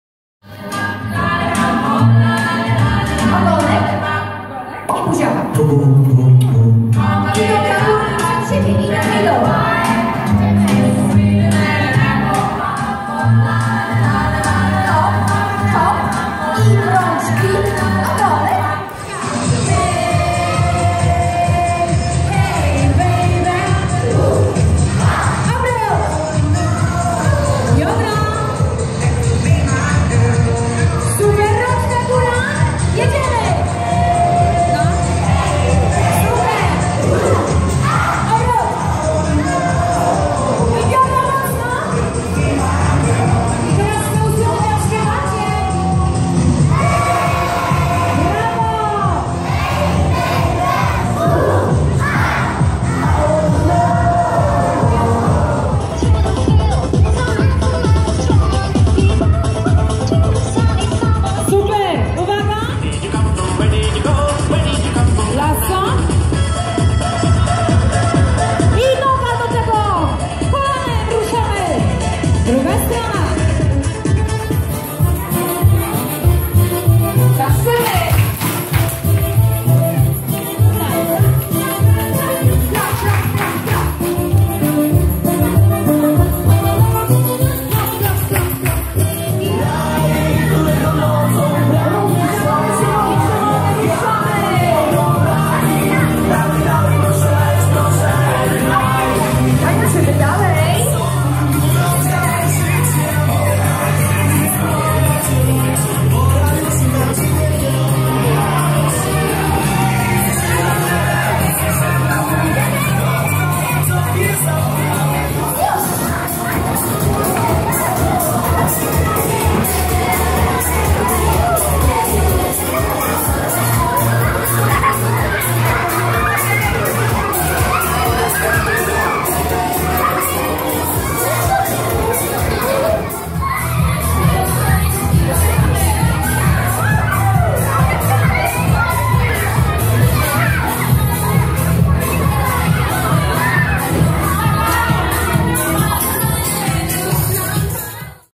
Karnawałowe szaleństwa z MDK KONIN
Bale karnawałowe prowadzone przez instruktorów MDK w Koninie.